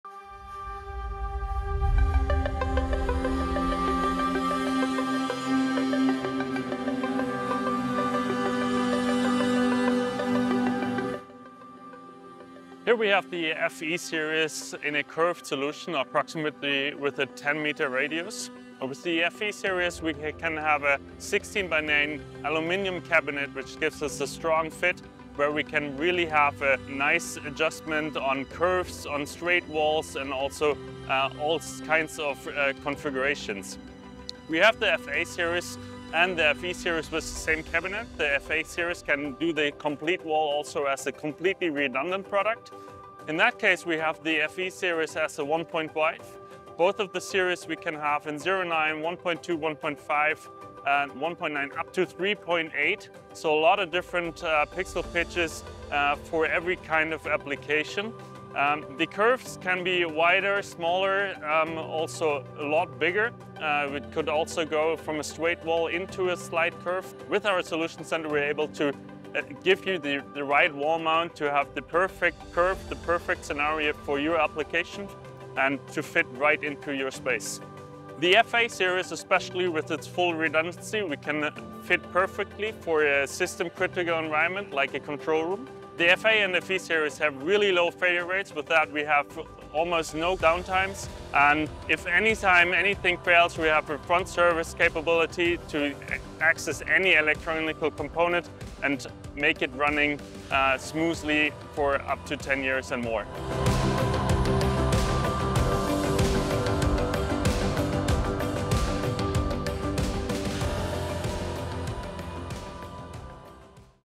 Sharp/NEC curved dvLED wall 10x5
With LED modules from Sharp/NEC, digital surfaces are no longer restricted to flat square displays. Create immersive experiences with a curved canvas offering unlimited scalability using customised mounting structures. Watch Nils Detje talking about the creative possibilities of LED technology from Sharp/NEC’s stand at ISE.